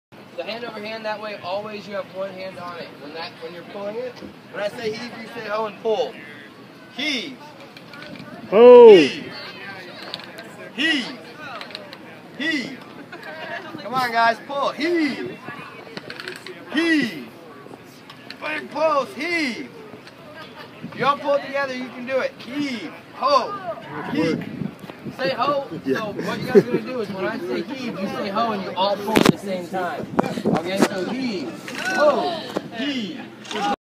Tall ships in Coos Bay, Oregon - kids raising the sail
My kids learning how sailors raise a sail.